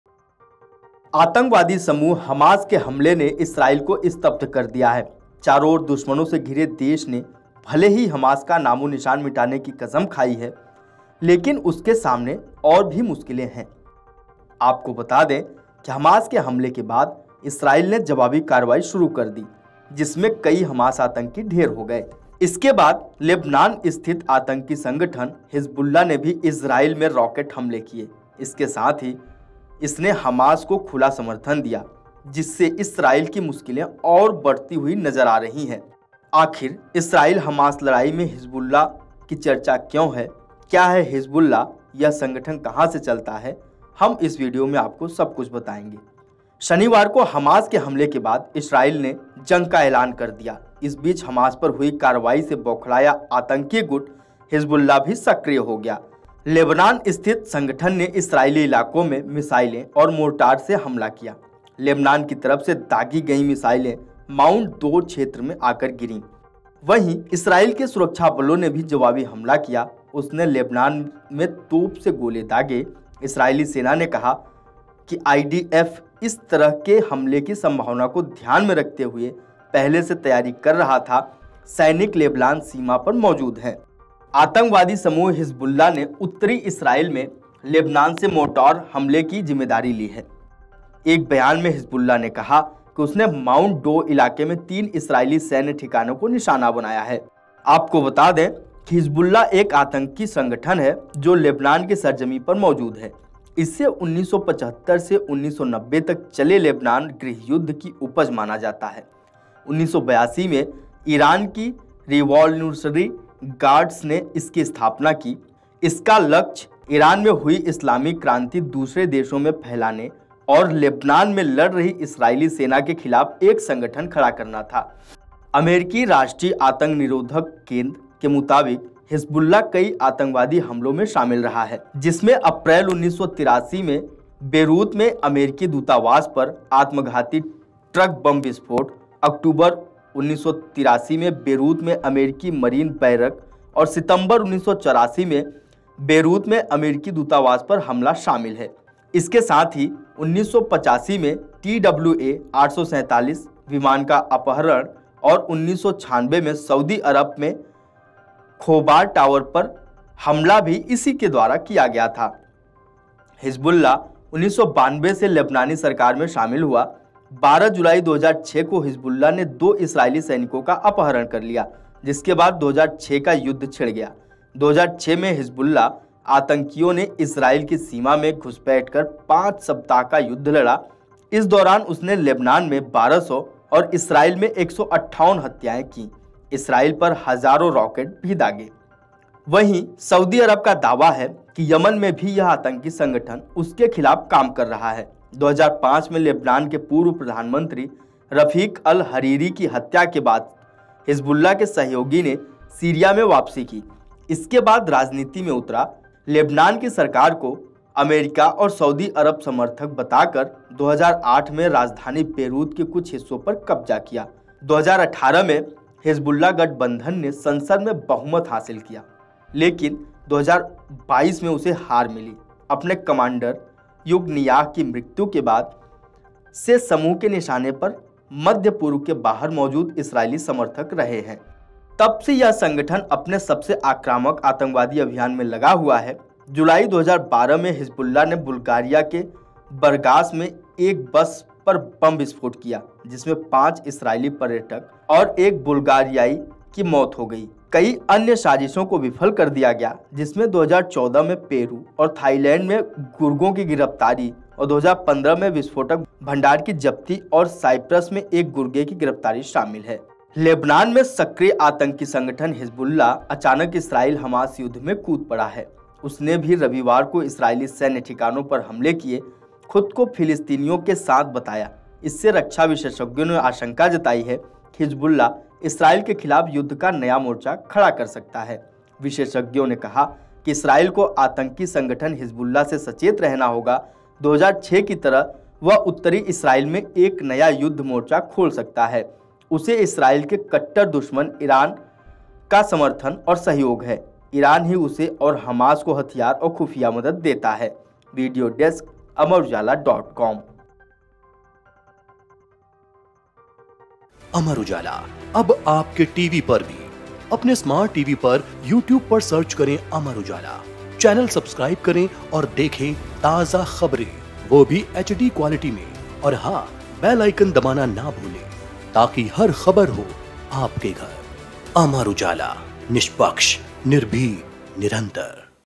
Israel-Hamas War: Hamas के बाद Hezbollah ने बढ़ाई इस्राइल की चिंता? US जैसे देशों में है बैन
लेबनान स्थित आतंकी संगठन हिज्बुल्ला ने भी इस्राइल में रॉकेट हमले किए। इसके साथ ही इसने हमास को खुला समर्थन दिया है, जिससे इस्राइल की मुश्किलें और बढ़ सकती हैं।